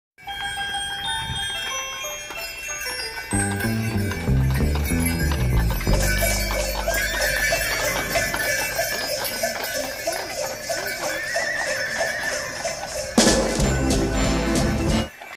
Sleigh bells ring through downtown Delano Minnesota 2021
The ReFoundry short for Small Business Saturday 2021